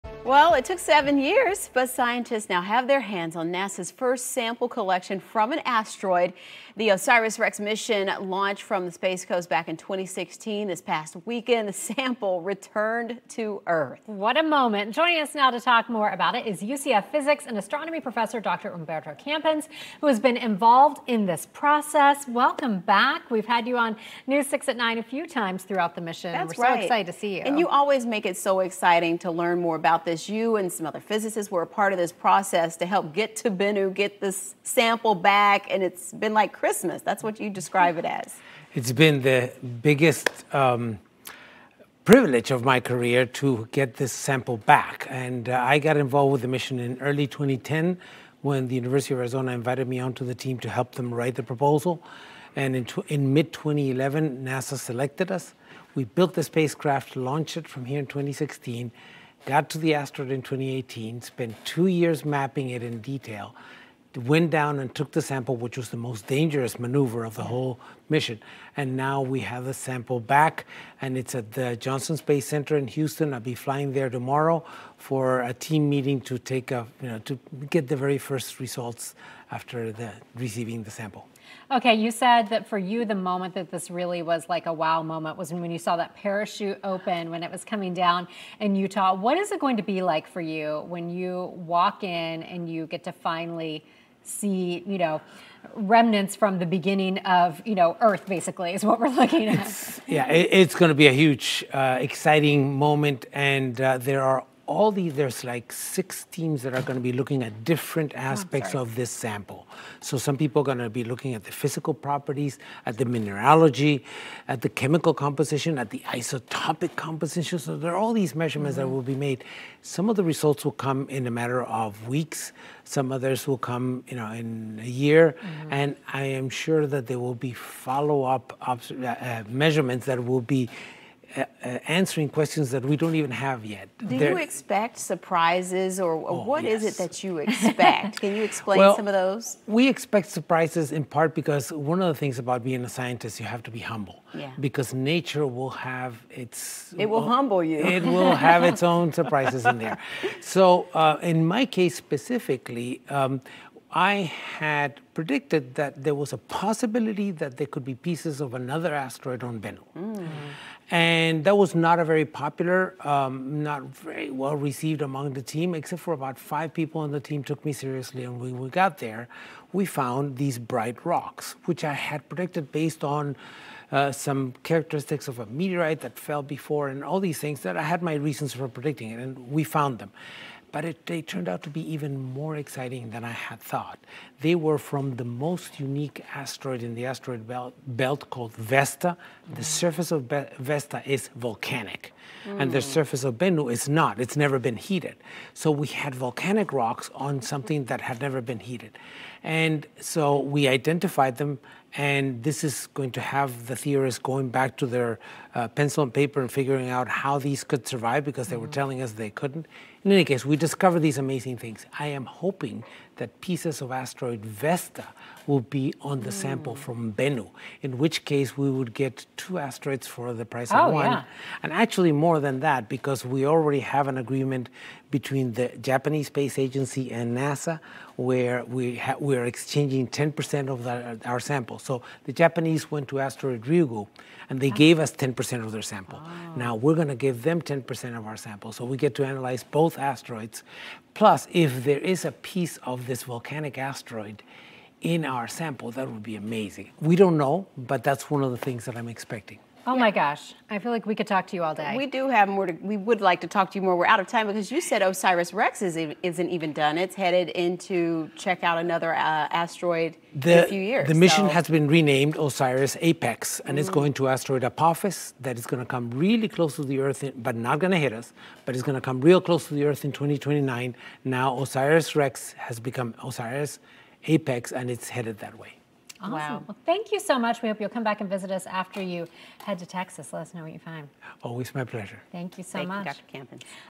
NASA's first asteroid samples land on Earth
UCF Professor of physics and astronomy Dr. Humberto Campins joins News 6 at Nine to discuss NASA's newly-retrieved samples of the asteroid Bennu.